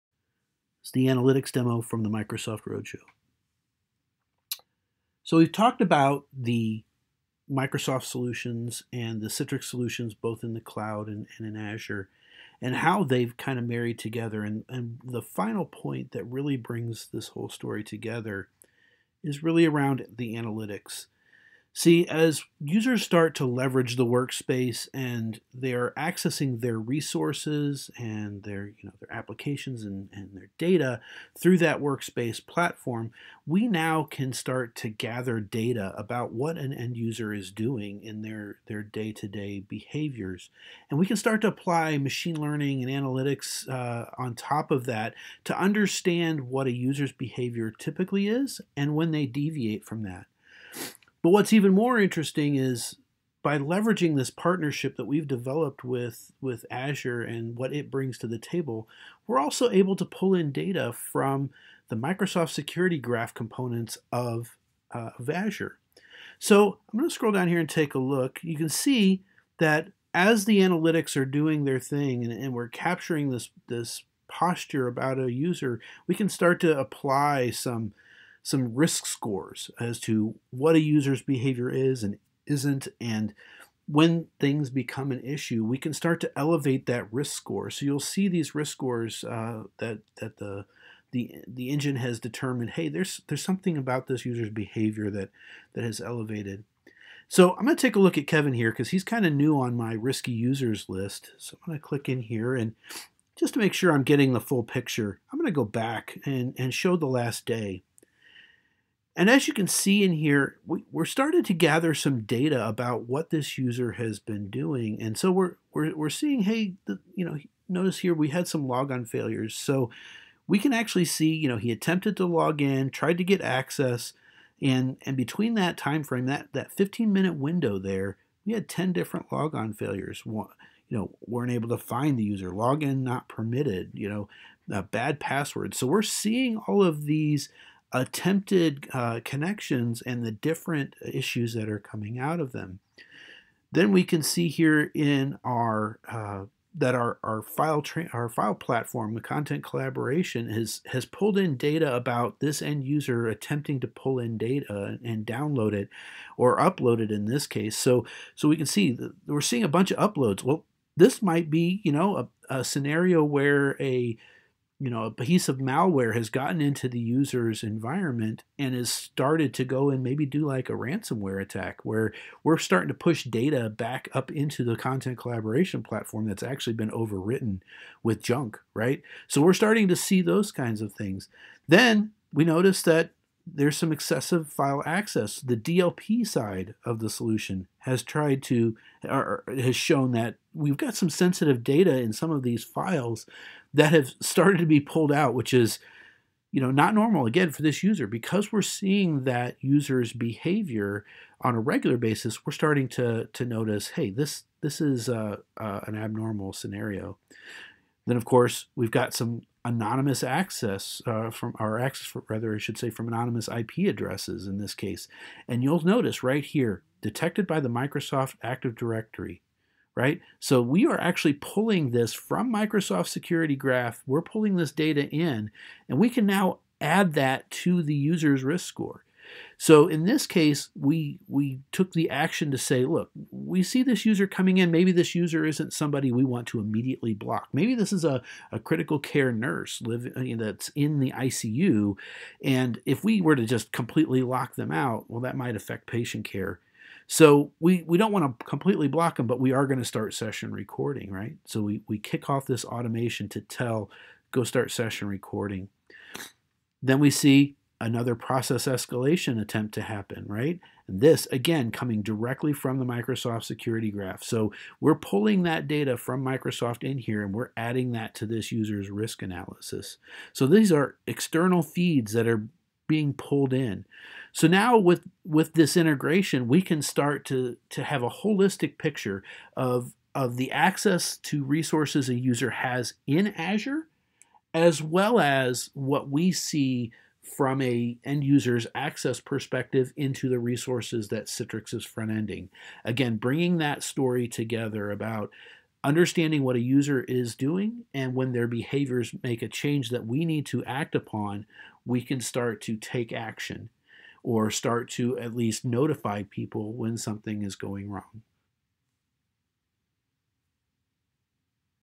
Citrix Analytics and Microsoft Intelligent Security Graph fuel the Modern Desktop
Utilize Citrix Analytics with Microsoft Intelligent Security Graph to achieve the future of AI-powered advanced analytics across the Modern Desktop